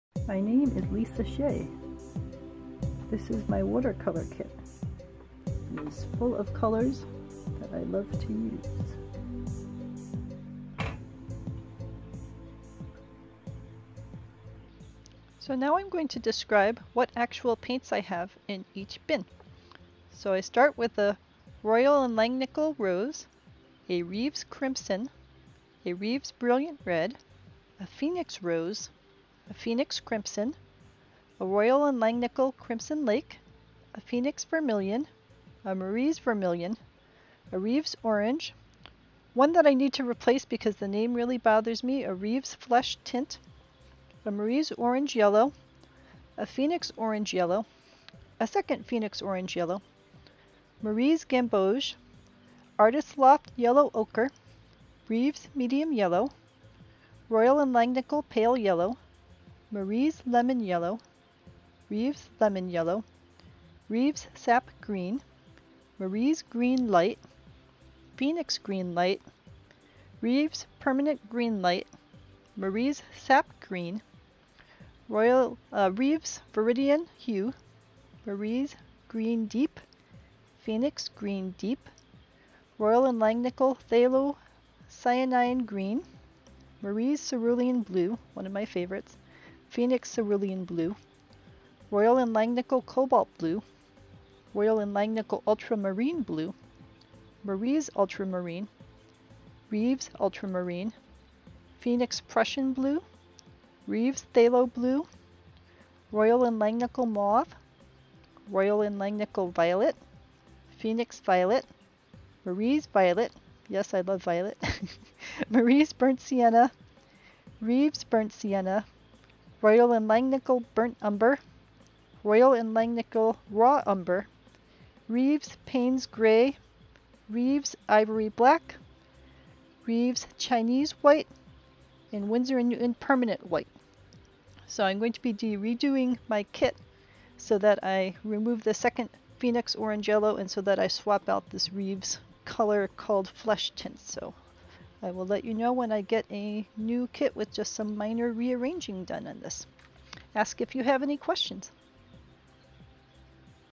My Watercolor Kit - Long Version with Names of Each Paint
This is the LONG description of my current watercolor kit as of March 2021. I use a FClub metal tin to hold the paints. The paints themselves are a selection of Royal & Langnickel, Marie's, Phoenix, Reeves, Artist's Loft, and Windsor Newton. All are fairly inexpensive and great for practicing with. I go paint by paint giving the name of each one.

Here's the short quick version which just shows the full palette.

Ask with any questions about my kit!